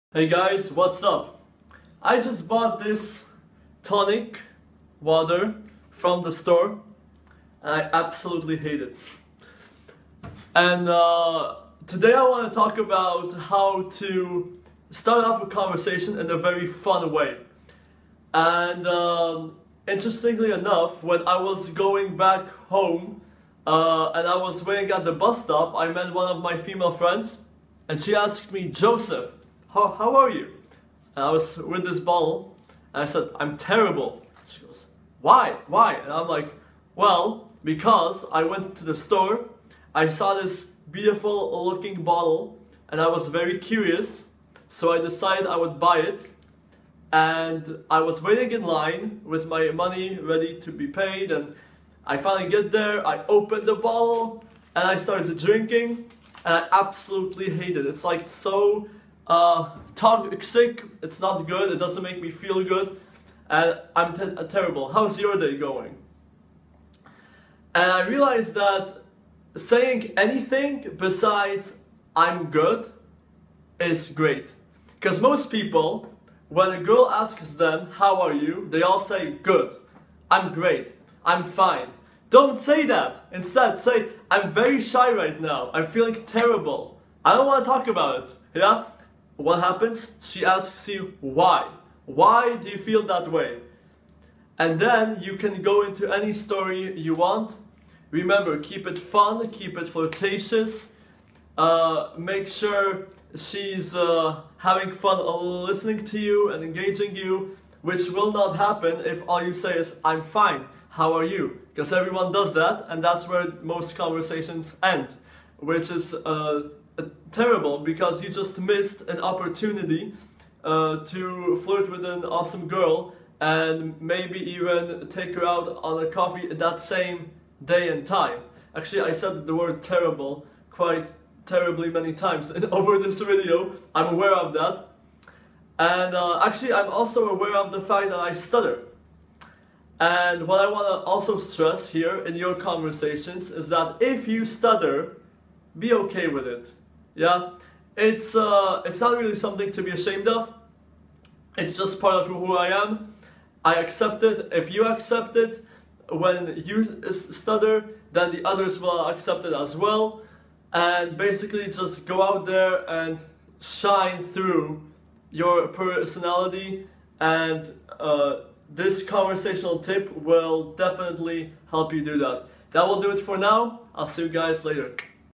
The Trick to Playful & Flirtatious Conversations
Most conversations I hear when men talk to women are boring and predictable. Learn this one tip to easily separate yourself from the sea of mediocre conversations and having a flirty vibe with hot girls.